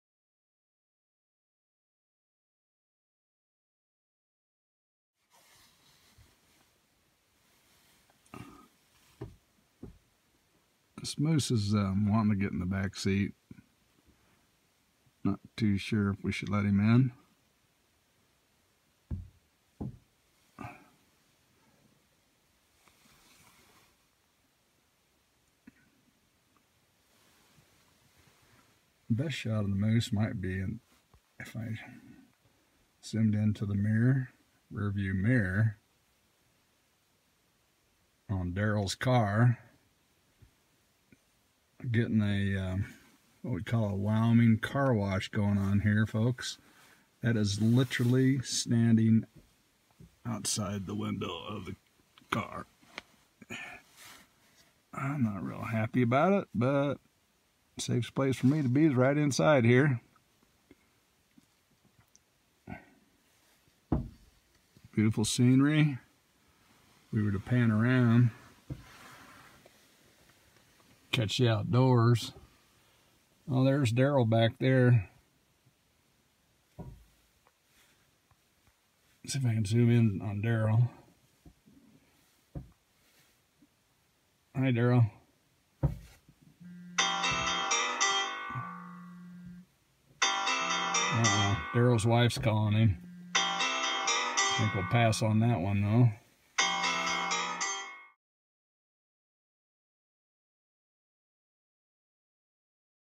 Bull Moose Hostage
While out looking for a great photo op today a bull moose got a little to close so I had to take refuge in the car!  Thanks Don Johnson for the last picture of me trapped inside.